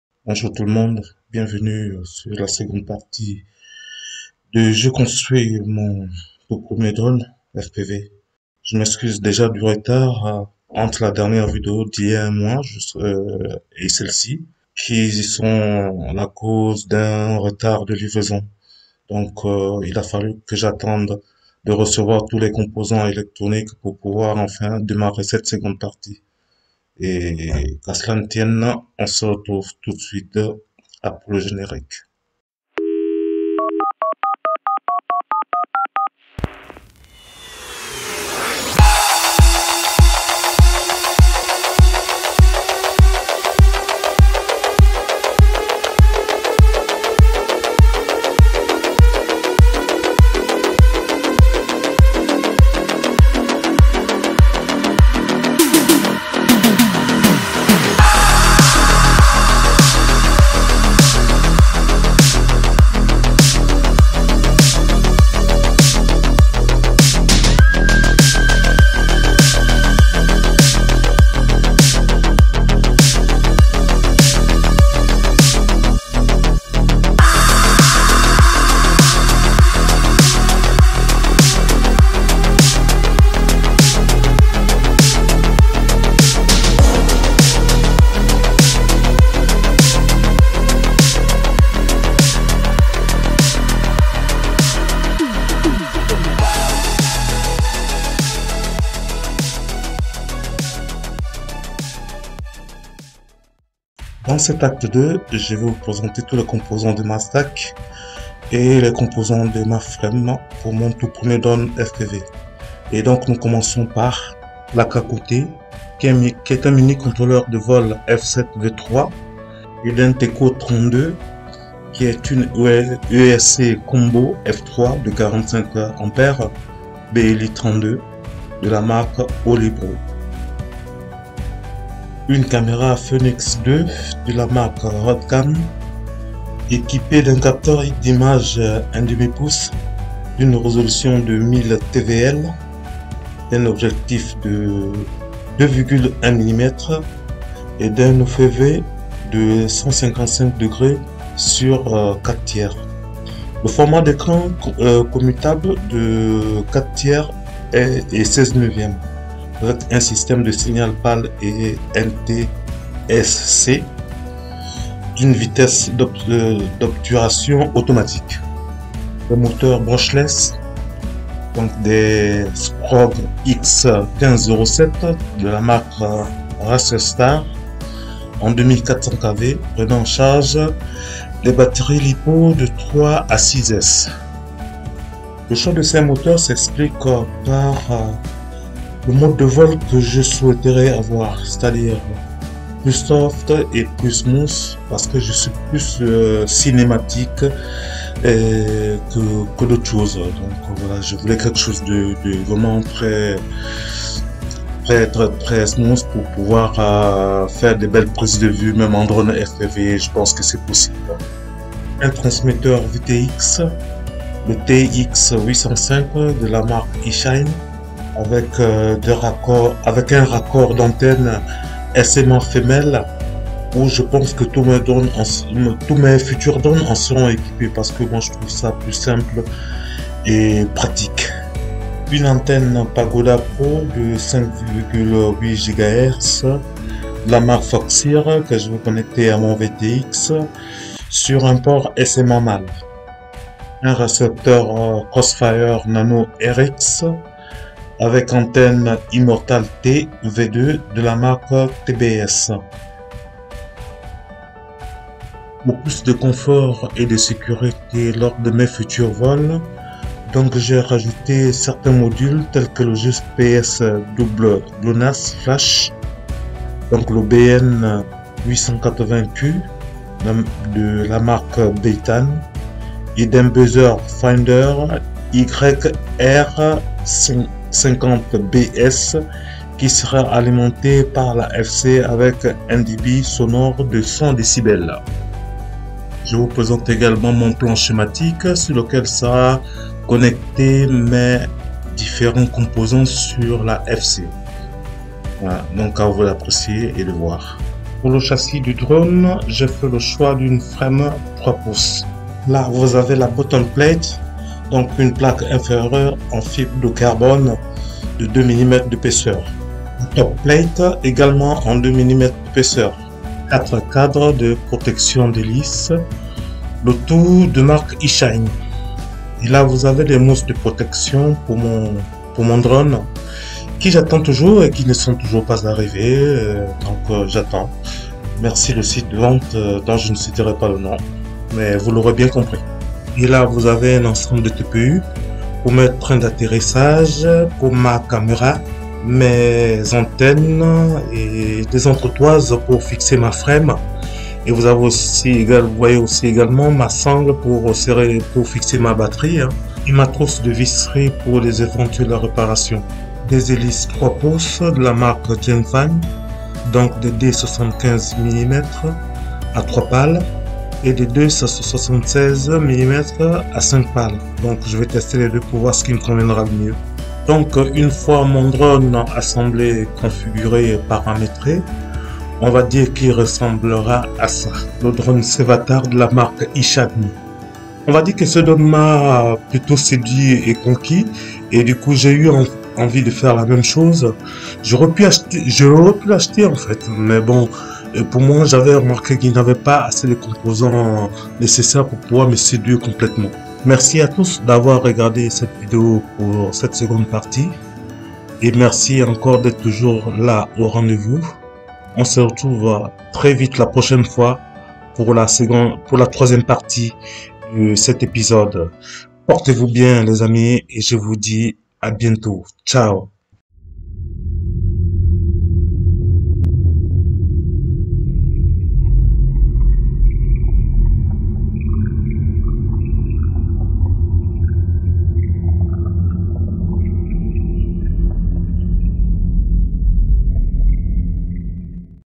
Tuto: Je construis mon premier Drone Fpv Act 2 "Choix des composants électroniques + frame"
En visionnant cette vidéo sur le choix des composants de mon futur drone Fpv, vous allez comprendre ma préférence du mode de vol. , Angle ?. Horizon ?. , ou Rate. !!! Je vous laisse découvrir.